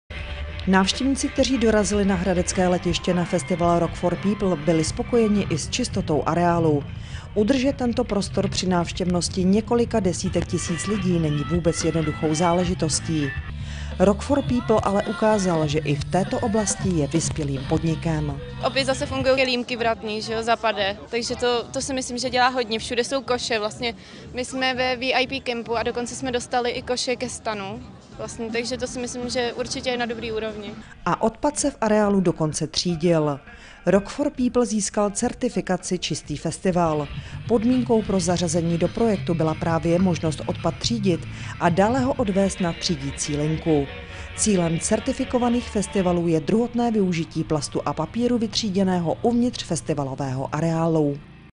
Rock for People čistý festival
Návštěvníci, kteří dorazili na hradecké letiště na festival Rock for people, byli spokojeni i s čistotou v areálu. Udržet tento prostor při návštěvnosti několika desítek tisíc lidí není vůbec jednoduchou záležitostí. Rock for Peple ale ukázal, že i v této oblasti je vyspělým podnikem…